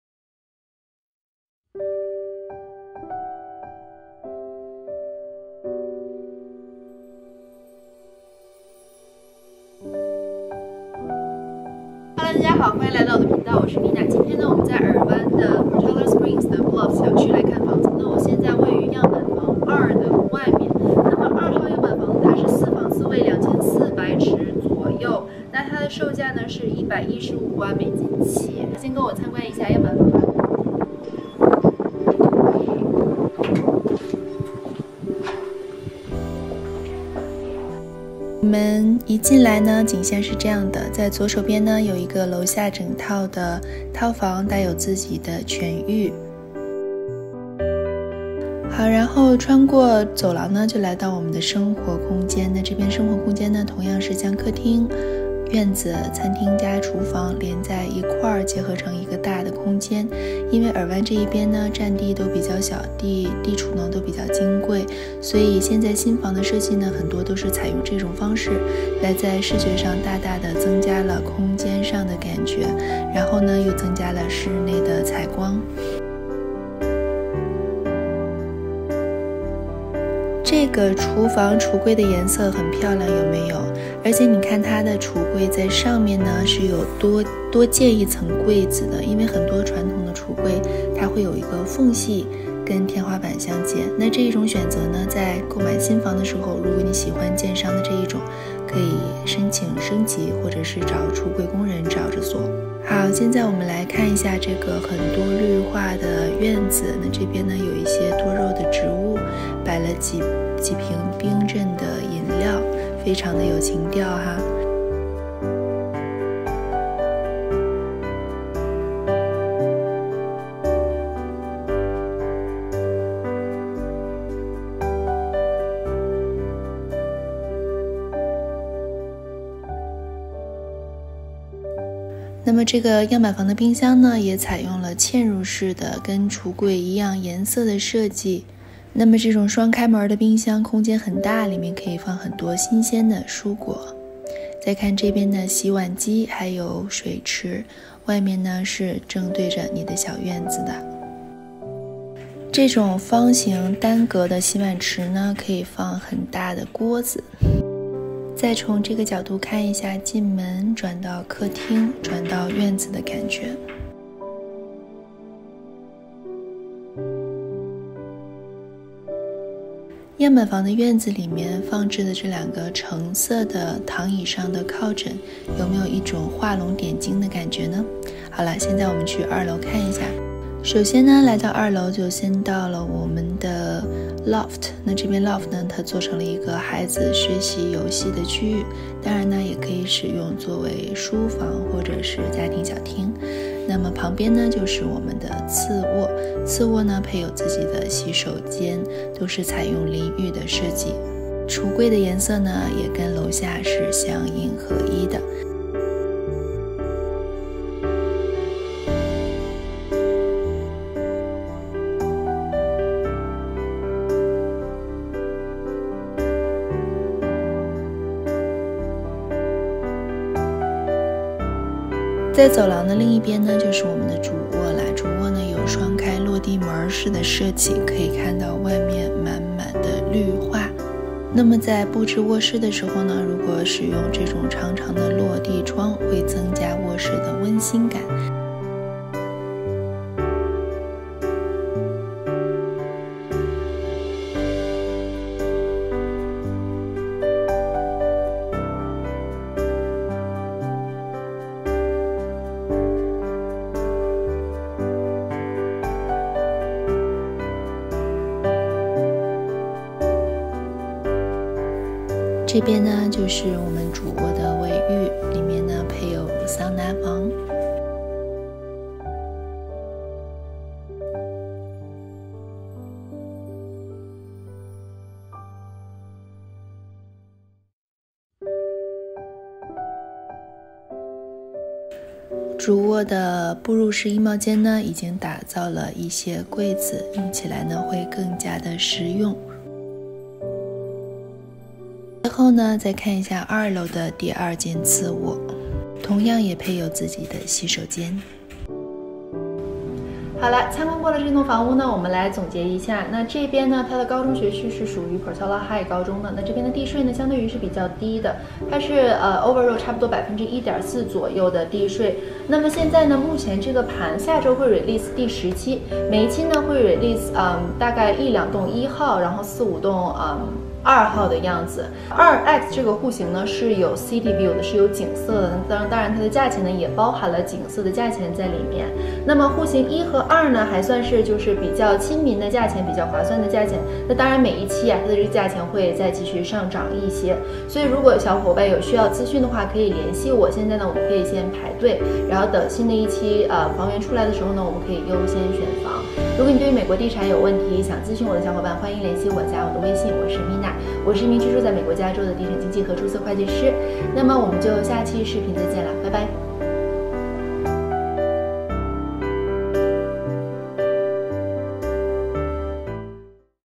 【新房开箱】 跟我一起看超人气布鲁富小区二号户型 | 高处俯瞰尔湾城景 | Bluffs小区户型2x | House Unboxing
BLUFFS @ 尔湾 Portola Springs 户型2x